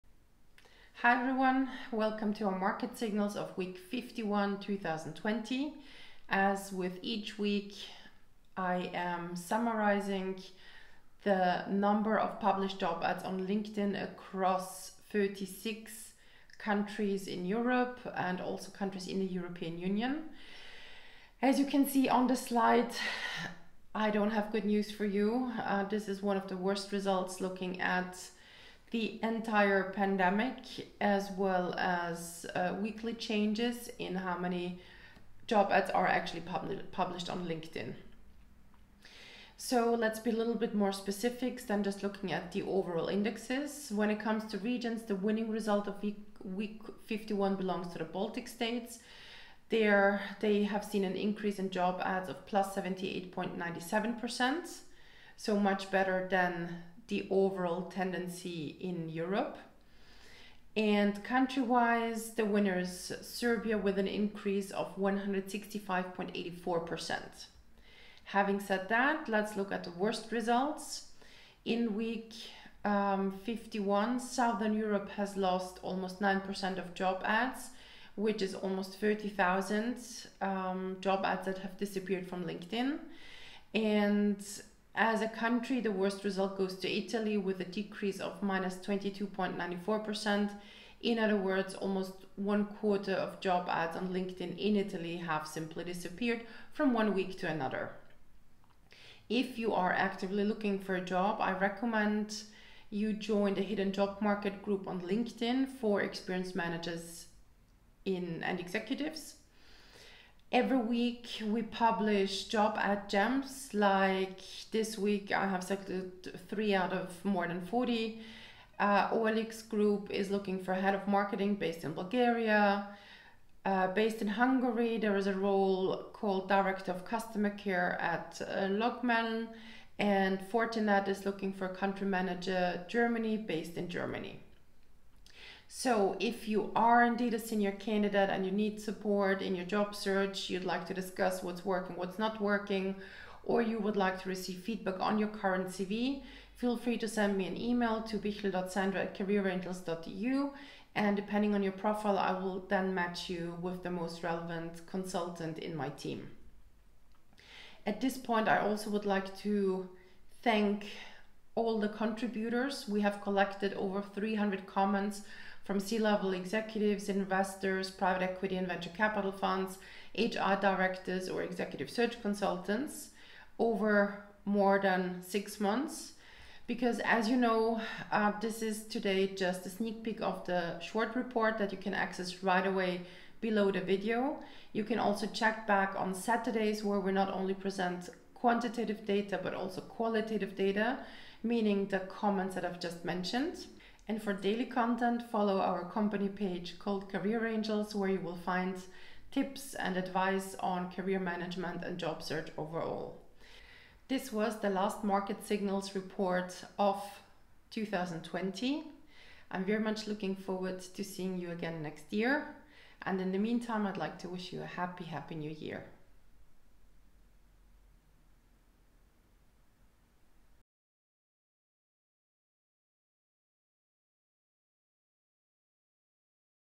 2020 weekly Linkedin job market report with job search tips for managers and executives [Week 51]
Market Signals of Week 51
Speaker: Sandra Bichl

Before there are trends, there are signals! One KPI that Career Angels track to observe changes on the job market is the number of published job ads on LinkedIn.

📈 Index 18, Index 36
🏆 Winners of the week: Benelux, Netherlands
👎 Losers of the week: Southern Europe, Luxembourg

Remember: keep applying - autumn is a good opportunity for candidates, as many companies need to hire experienced managers & executives to start work at the beginning of next year!

Job ad gems of this week:

OLX Group

LogMeIn

Fortinet

The full list on the "Hidden Job Market for Experienced Managers in Europe" Linkedin group:
← simply request access to the group!

Career Angels is an international career consulting boutique that provides tailor-made services to Top Managers & Directors since 2010.